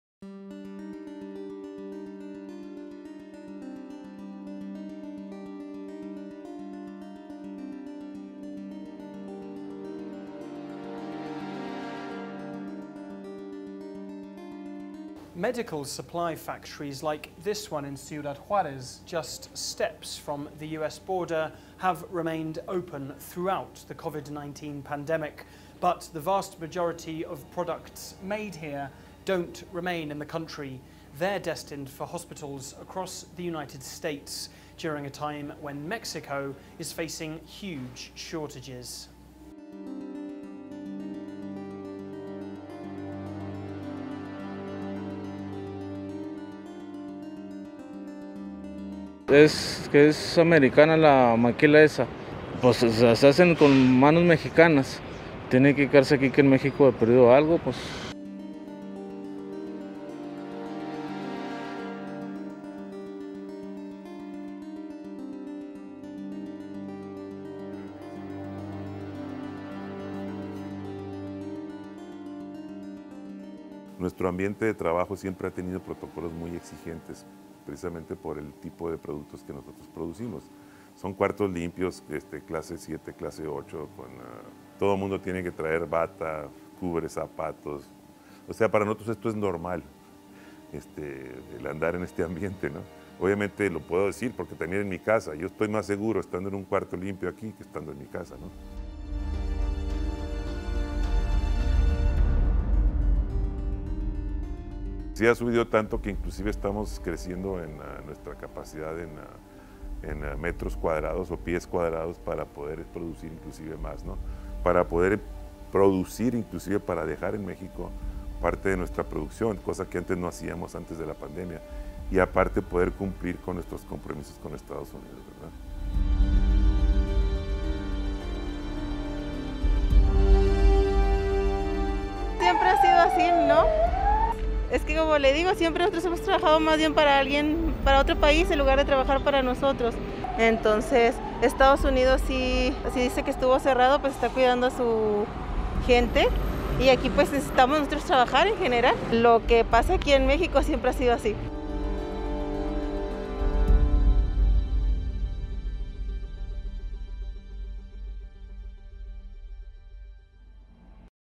Mexicans are making medical supplies they can't use
Medical supplies like face masks and sanitizing gel are in high demand ​amid the ​COVID-19 pandemic. But in some less-economically powerful countries, they can be difficult to obtain.

Mexico, for example, has faced chronic shortages of most products. Yet when it comes to ​their manufacture, the country is booming. CGTN's Alasdair Baverstock reports.